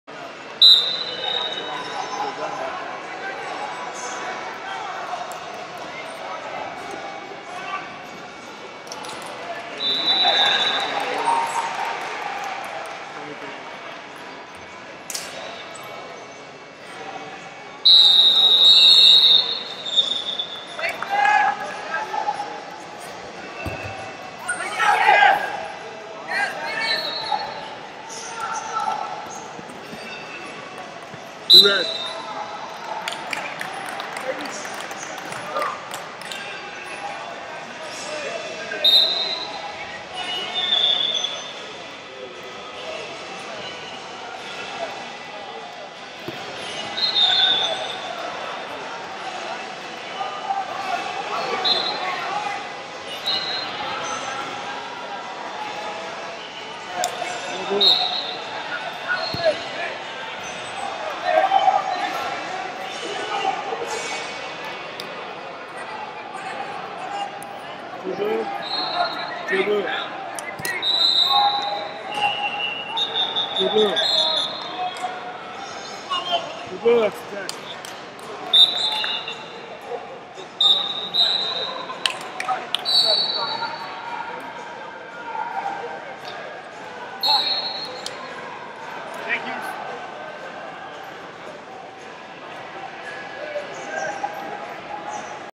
74 Kg Consolation - Will Esmoil, Iowa Vs Jarrett Jacques, Tiger Style Wrestling Club 8b6f
FloSports/2021 UWW U23 Nationals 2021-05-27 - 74 Kg Consolation - Will Esmoil, Iowa Vs Jarrett Jacques, Tiger Style Wrestling Club 8b6f